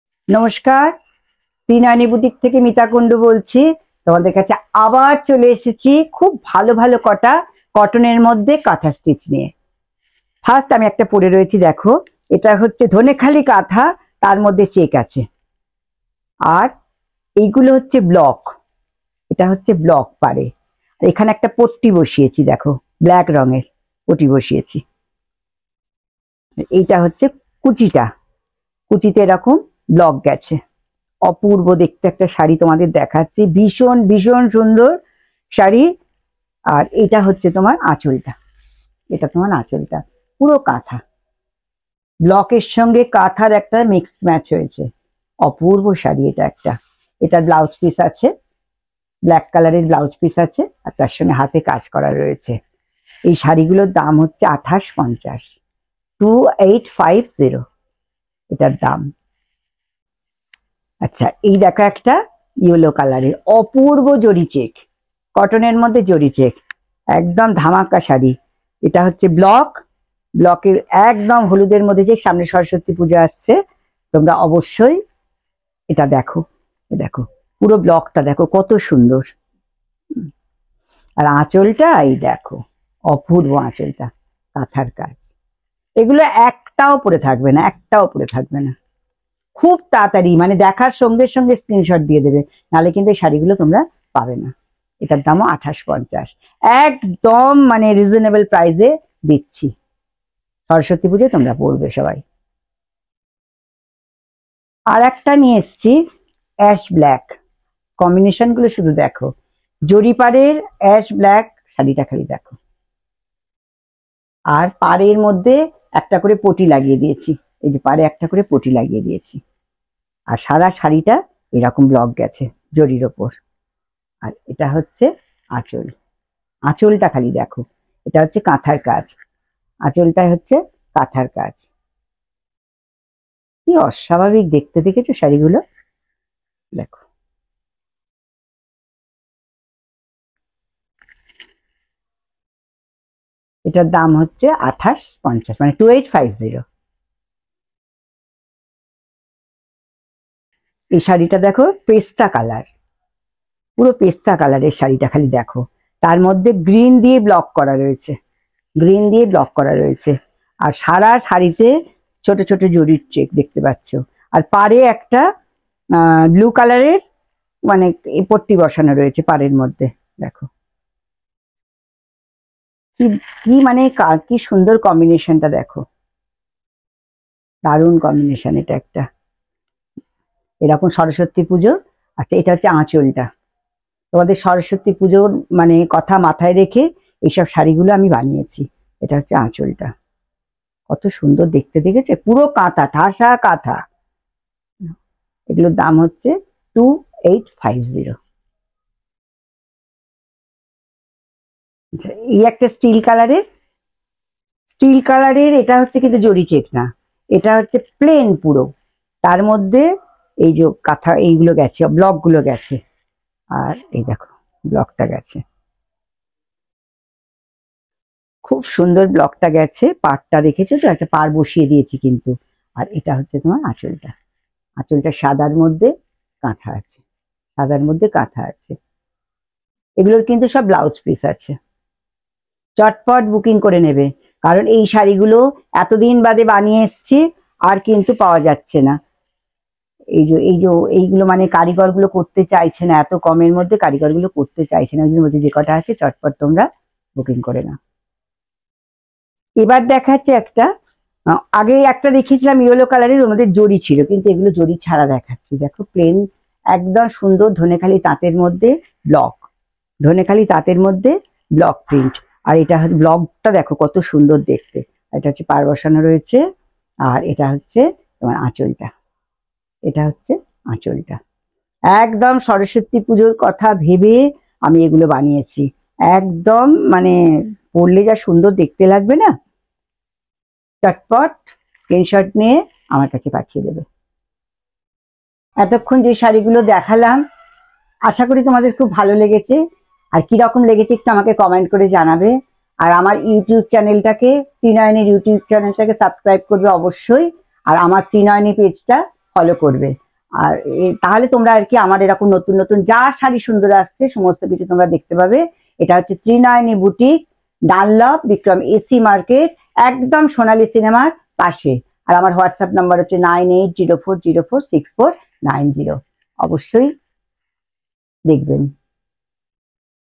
Traditional Kantha Stitch Sarees | Kantha Stitch on cotton
"Handcrafted elegance: This Kantha cotton saree is a canvas for artistry, where needle meets fabric to create a masterpiece as unique as the woman who wears it."
👉 Our Branches Addresses - Dunlop Bikram AC Market Shop no.- 32